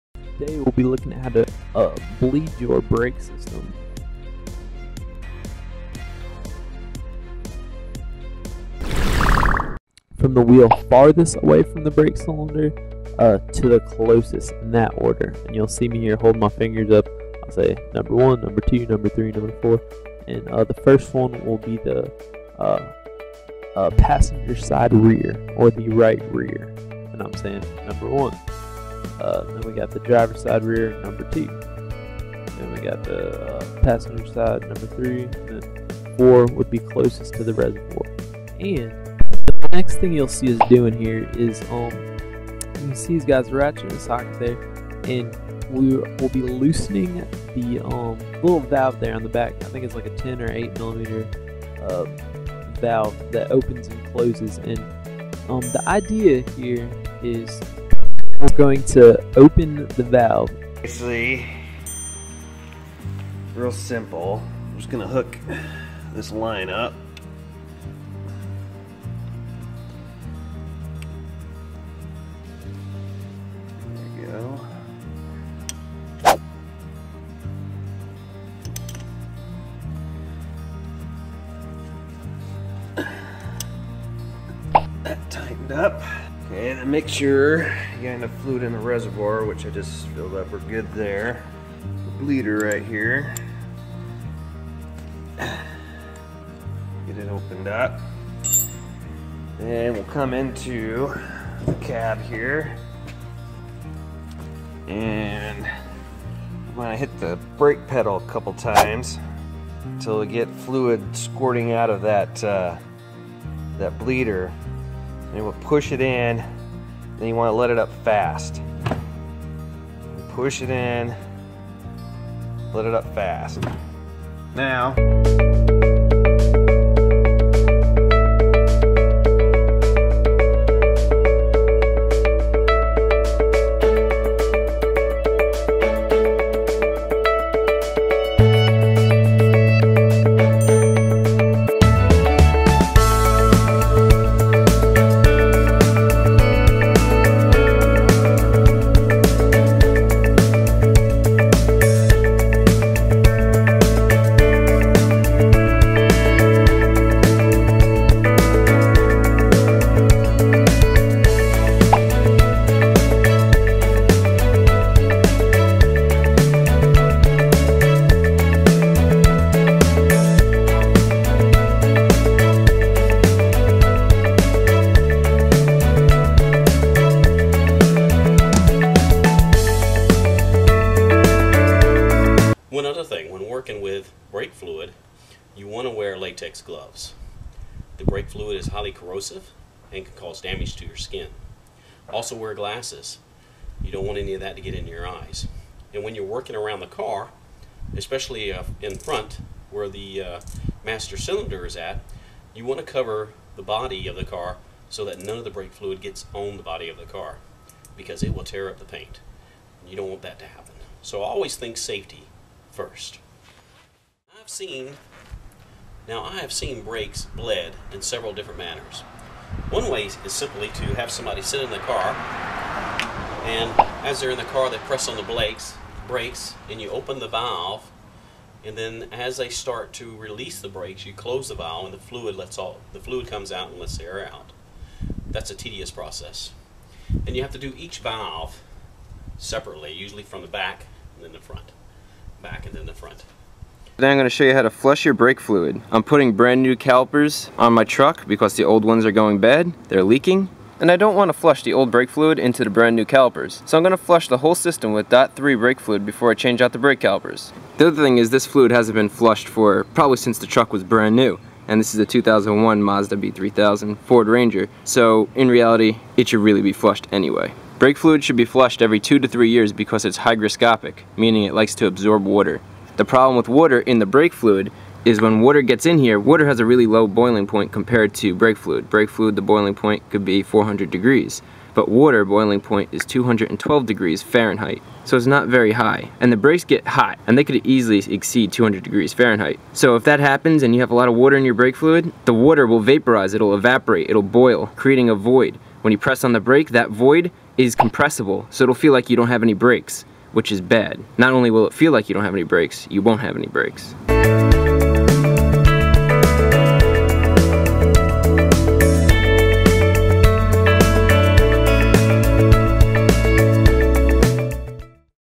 How to bleed brakes
In this video, shows you how to bleed or purge the air from the system by yourself

Source Video!